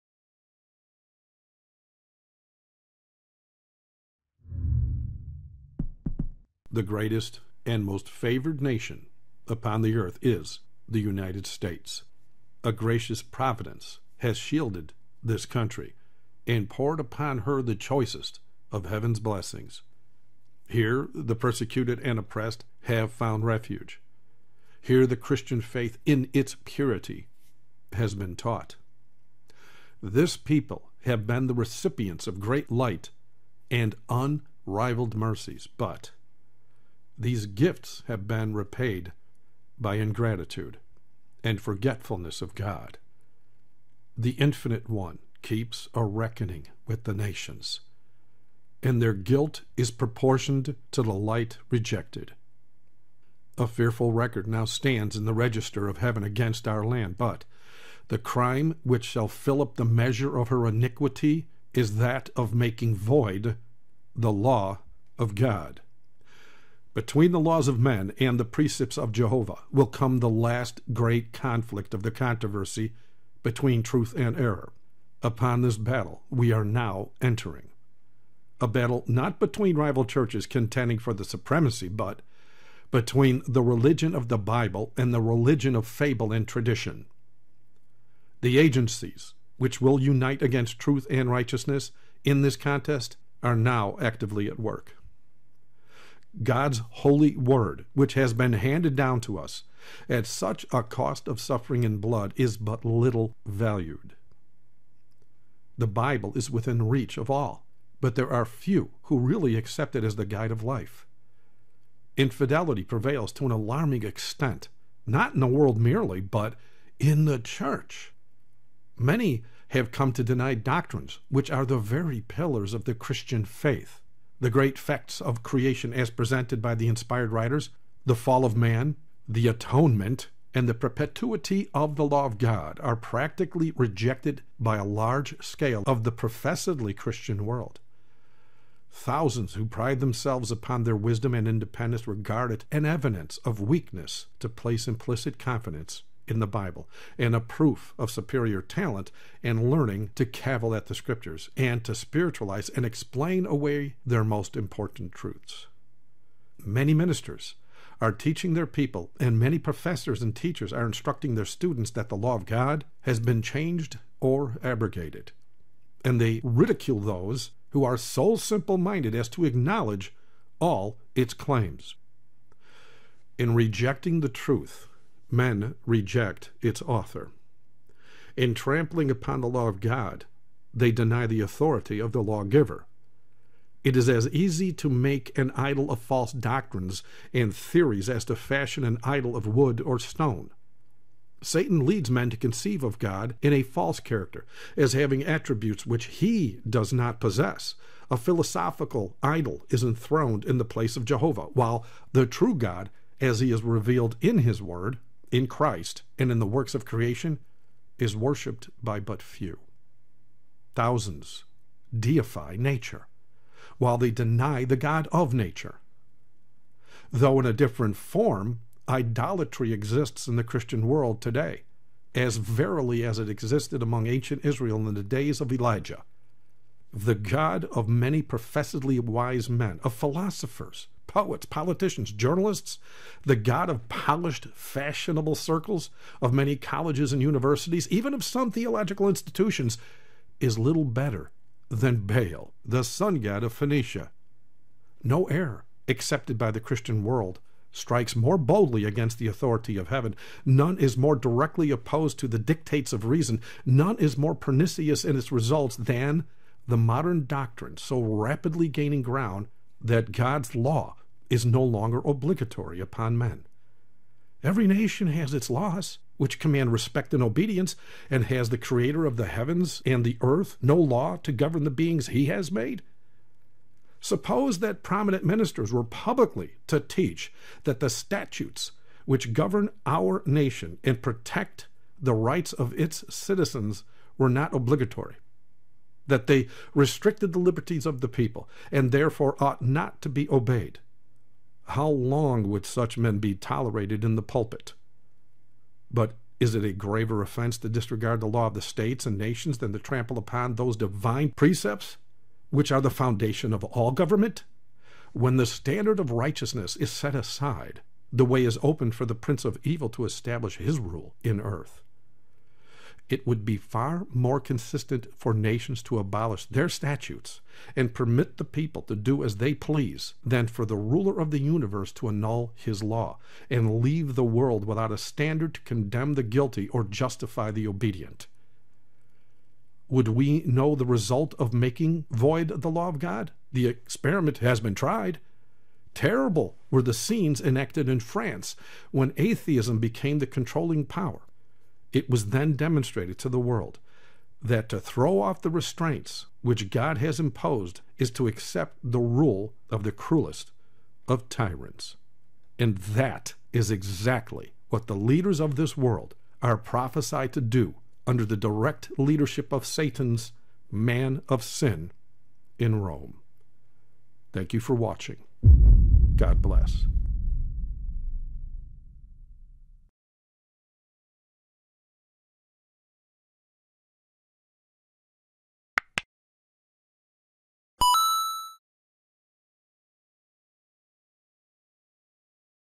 The Coming Conflict
I share a very small portion of the Spirit of Prophecy volume 4. It's happening RIGHT NOW!

If you don't already have this book, YOU NEED to get it ASAP. It contains many prophetic facts that for the most part have already been fulfilled.

PLEASE SHARE THIS WITH MANY! Countless souls are on the precipice of eternal life and death as we speak.

All links to prophetic and historic info mentioned in this video are listed in my poGm blog entry